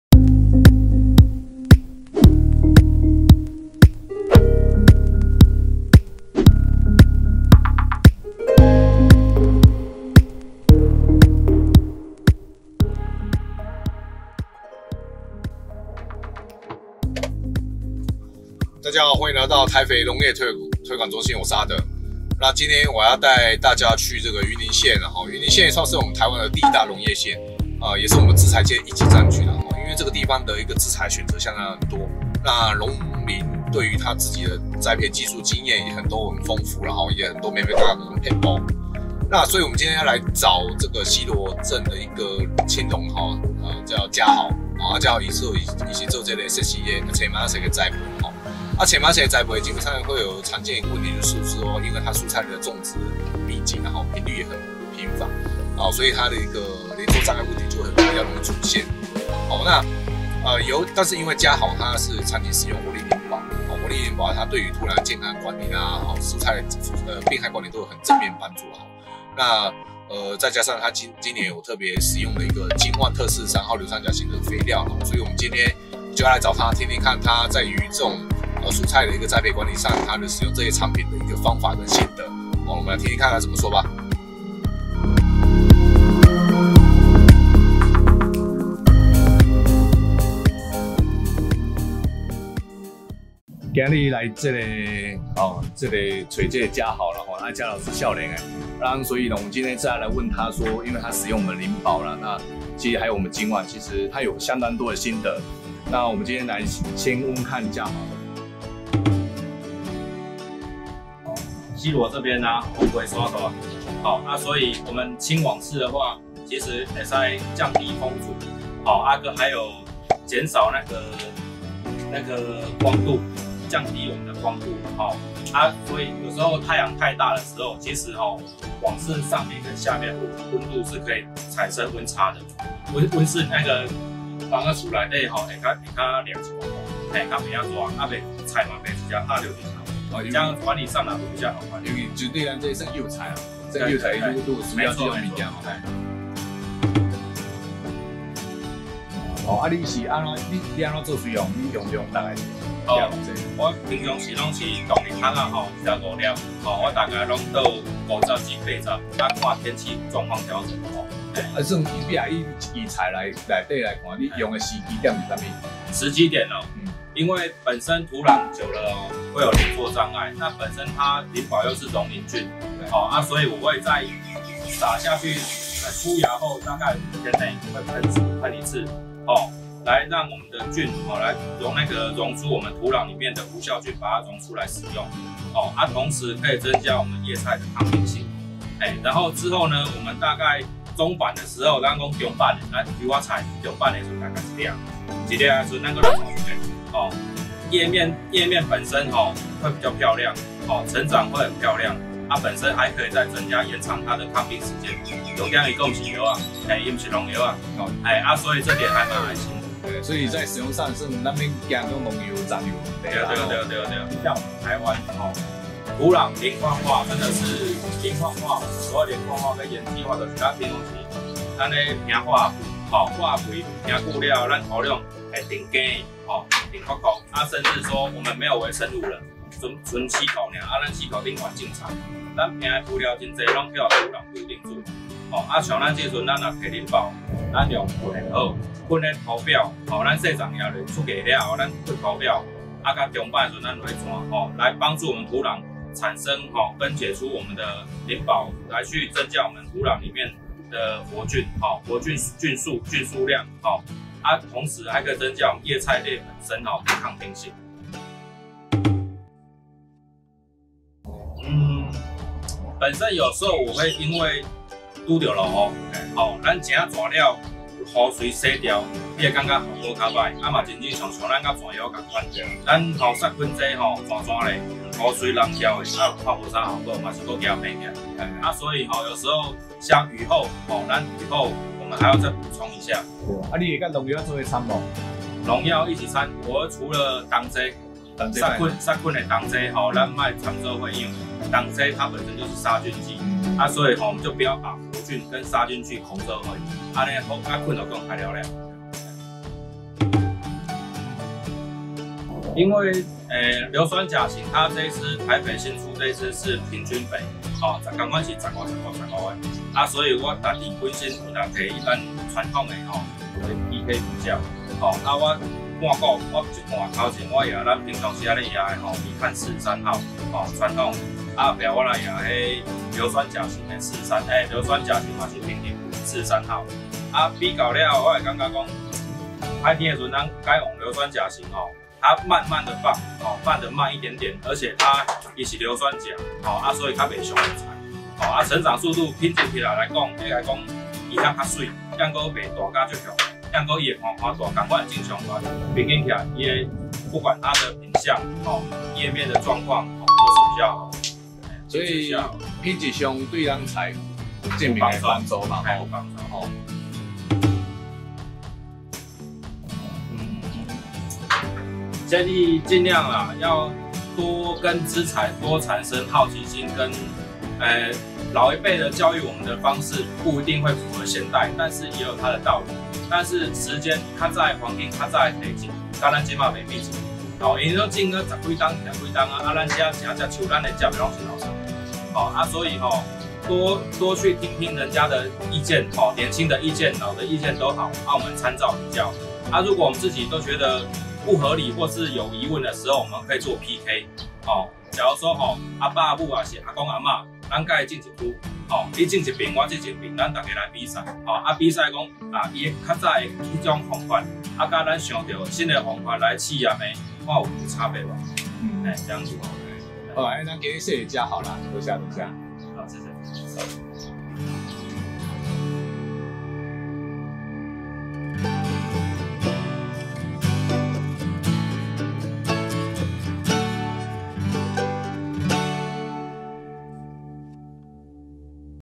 台肥活力磷寶與金旺43號怎麼使用?西螺青農分享白菜肥培應用
台肥活力磷寶是屬於高菌量微生物肥料，對於蔬菜栽培土壤管理上有正面的幫助，另外台肥公司新產品硫酸鉀型金旺特43號亦可以幫助提升葉菜類作物品質與延長儲藏性，我們來聽聽西螺青農家豪怎麼說吧!